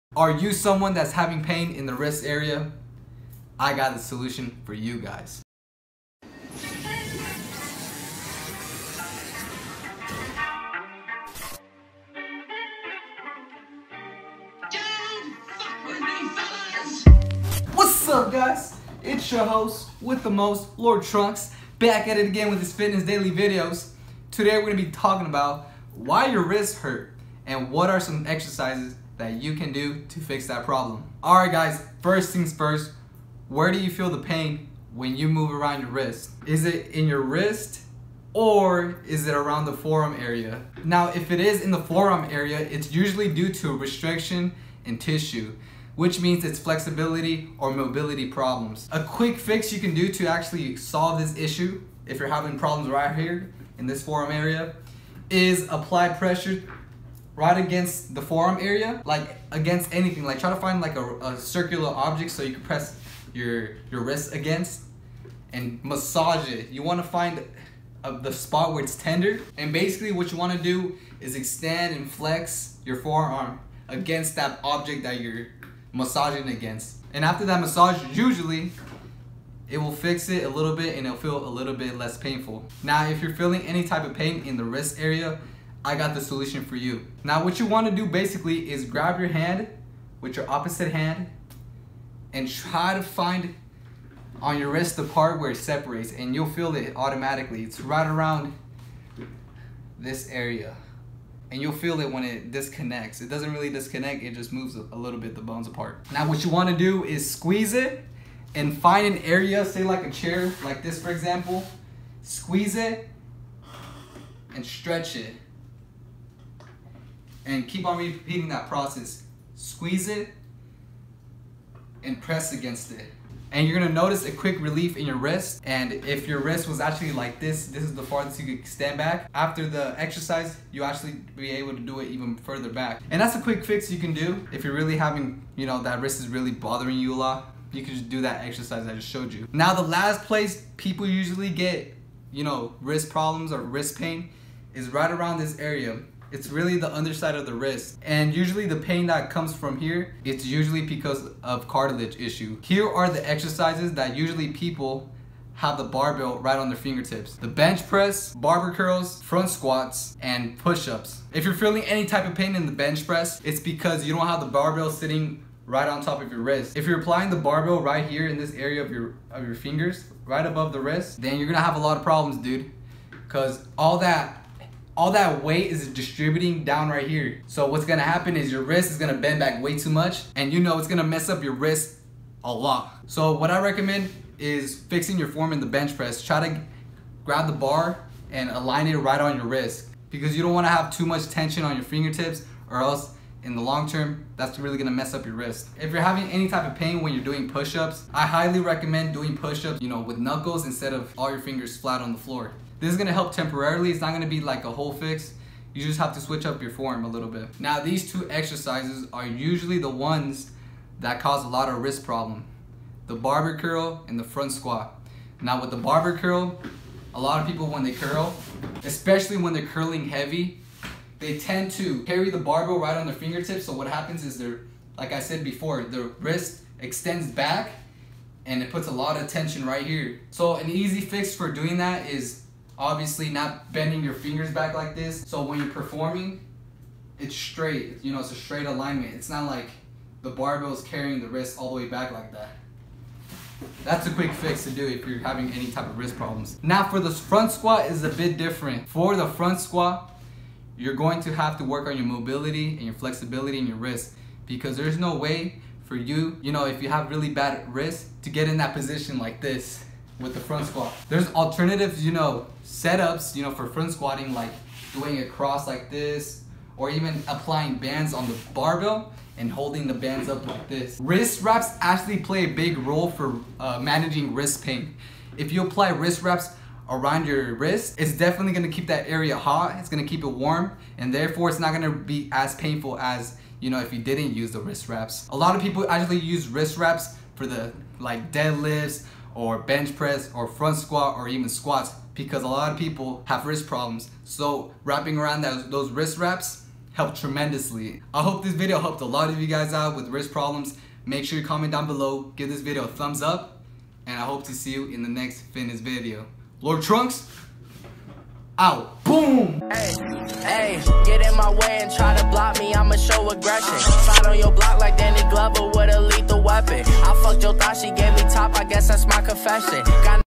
How to Fix Wrist Pain!
if you're experiencing any type of wrist pain when you're working out. This video is perfect for you! in this video, i'm going to show you what's causing you're wrist pain and how to minimize aching,tenderness, or soreness in that area.